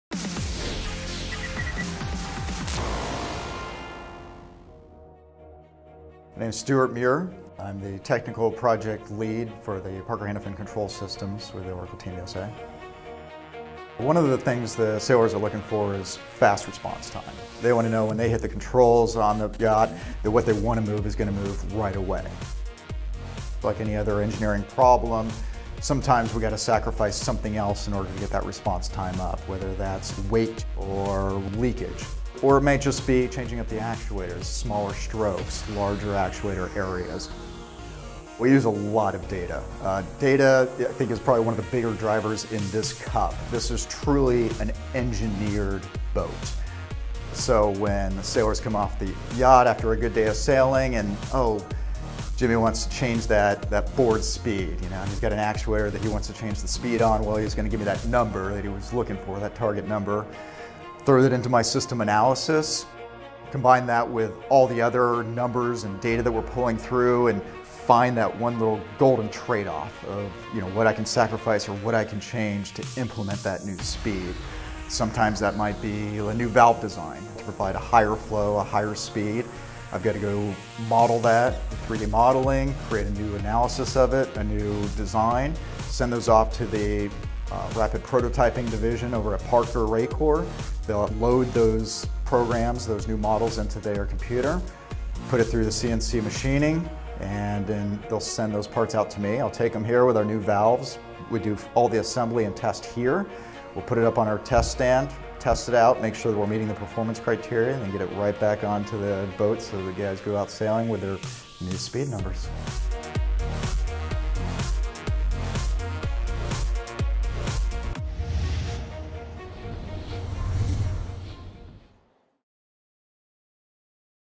TechTuesday: Fine Tuned for Fast Response
TechTuesday: Fine Tuned for Fast Response. We want to know that when we hit the controls, the yacht will quickly respond. Stuart Meurer, technical project lead of our control systems with Parker Hannifin, explains how we do exactly that in this feature by John von Seeburg & Mike Herbener / ORACLE TEAM USA.